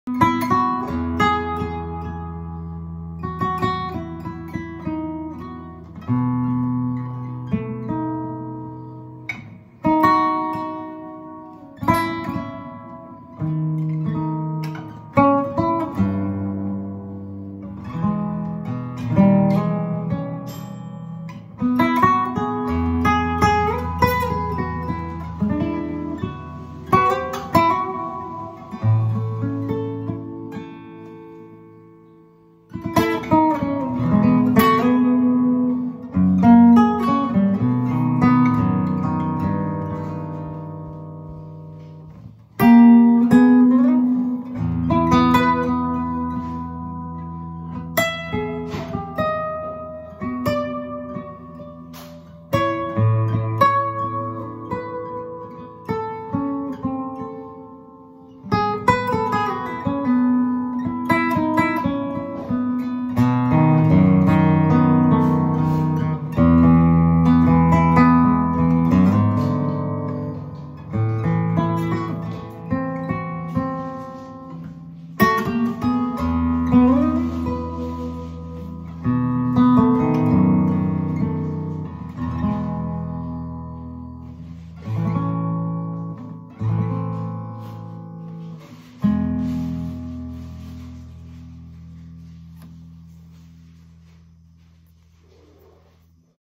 Takamine TC132 SC
Premesso che nessuna chitarra potrà mai mitigare il grande zappatore che sono posso considerarmi davvero felice di questo nuovo arrivo a casa. Non me ne vogliano i chitarristi seri che usano le dita e le unghie, in questo caso il mio DevilPicks in cocco e il fido Lombardi Giallino mi regalano un sabato qualunque come fosse già Natale. Sopratutto hugs & thx to David Pulkingham for help me to choose this gtr. YOU BEST, my friend! 💪🏼💥🚀🍷❤️